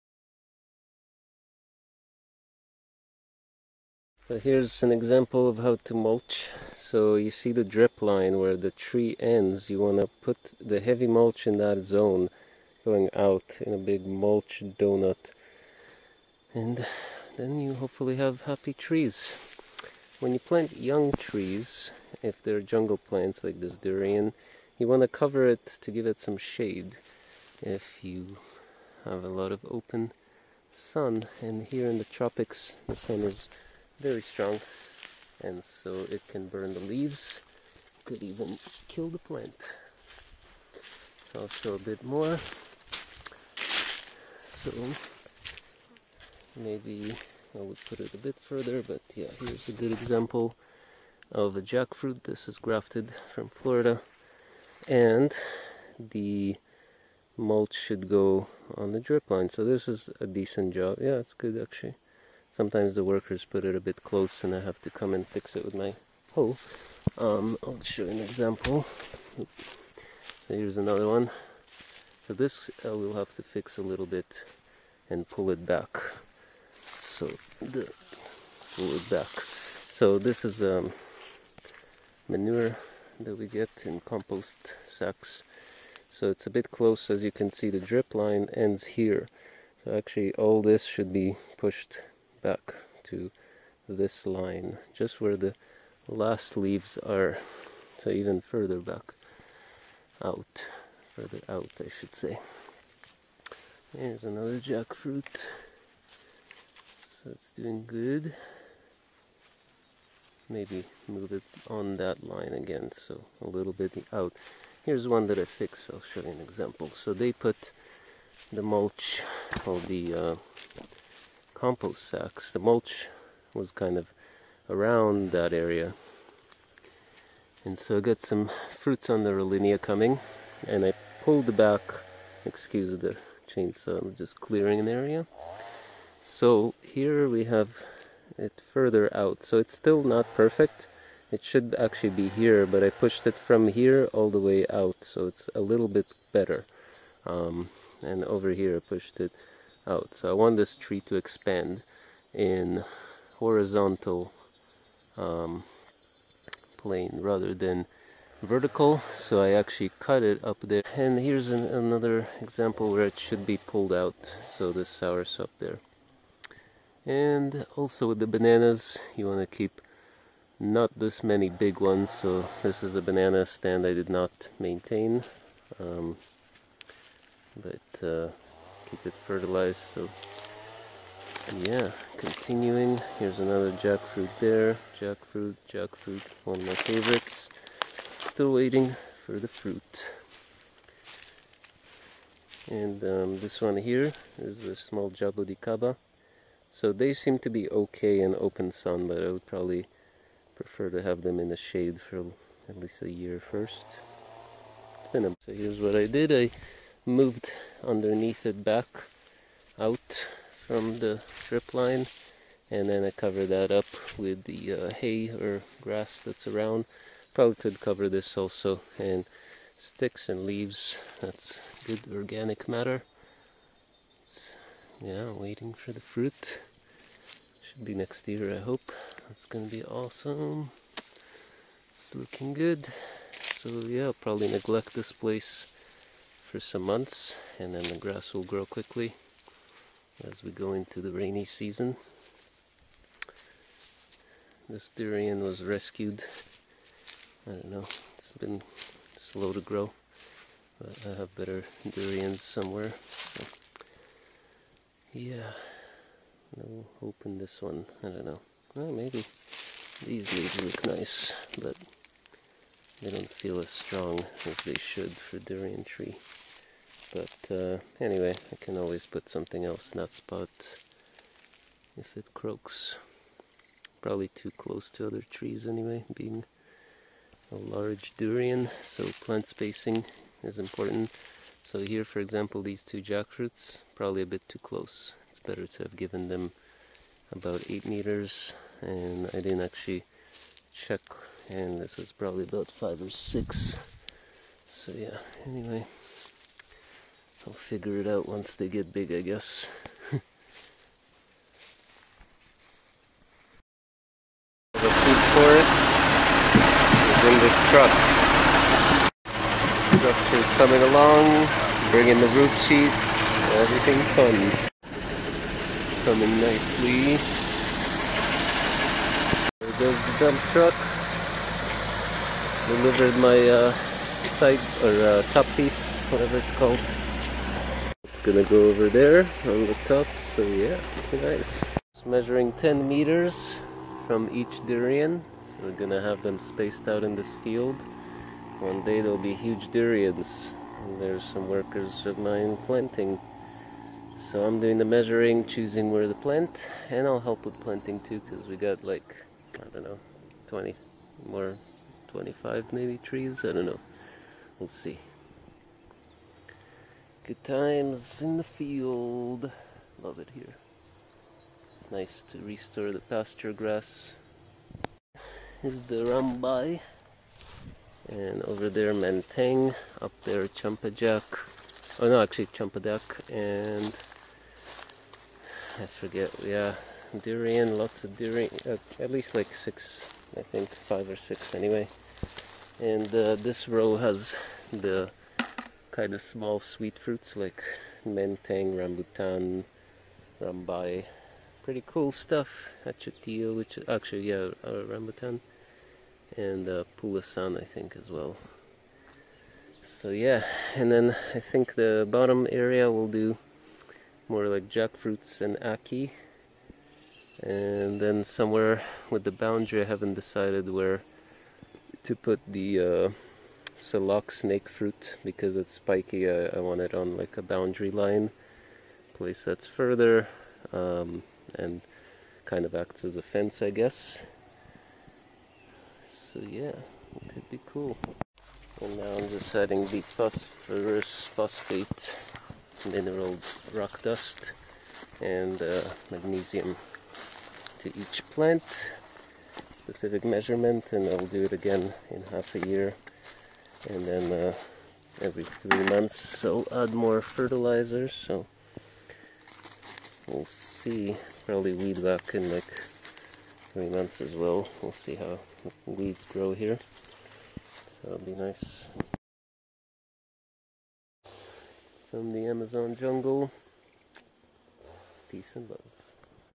Maintaining, planting and building at Fruit Haven Ecovillage Ecuador personal lot
At my personal lot doing some maintaining of older trees, planting new trees and building a new structure!